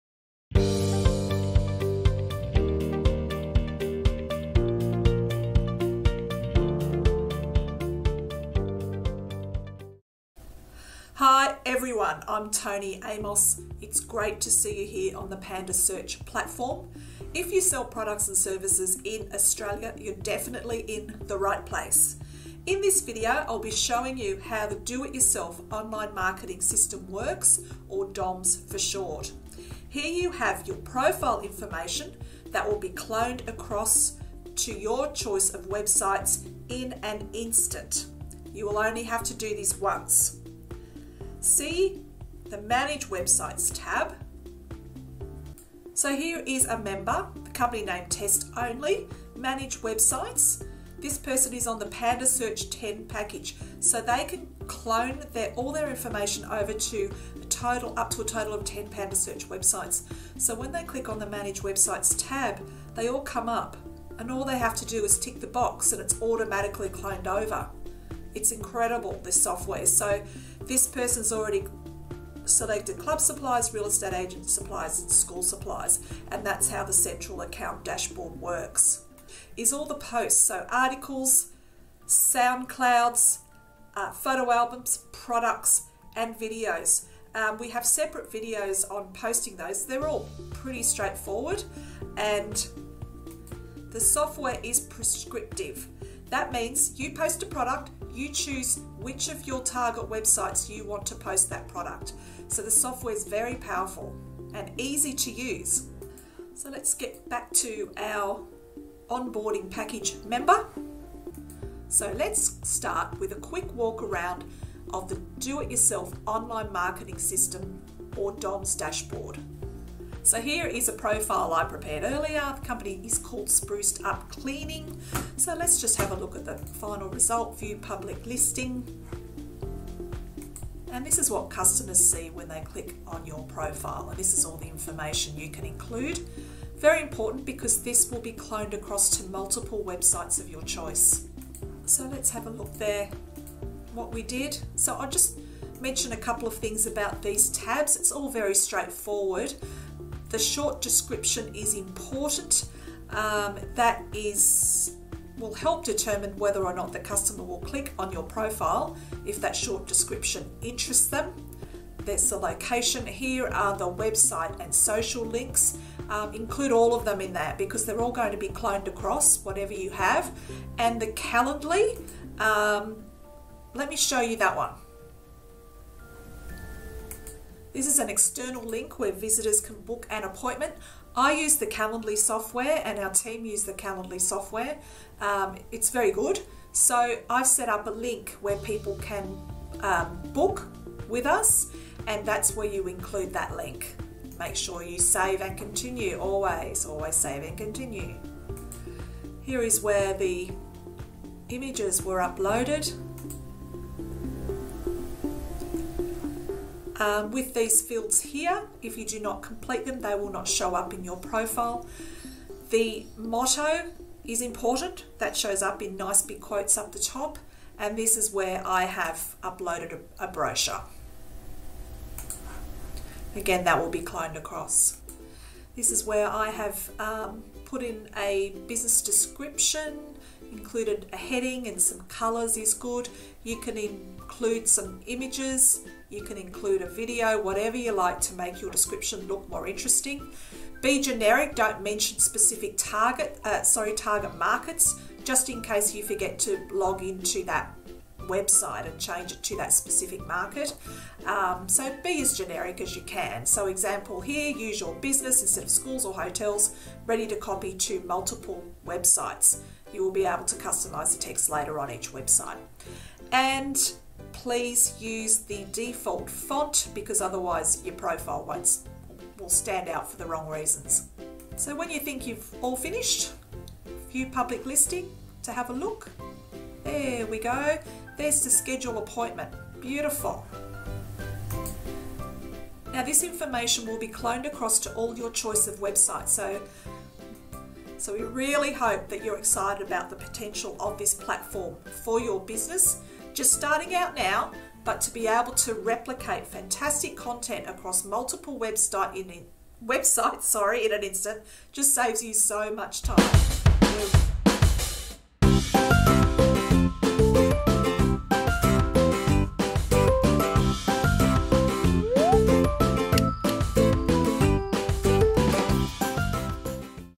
Onboarding Video Tutorial - Advertisers on Panda Search
This video shows sellers or advertisers what to do once they sign up to the Panda Search platform.  The onboarding process is quick and easy and includes setting up your Profile, social media links, website links and business location.  Go to PandaSearchAdvertising.com.au to sign up.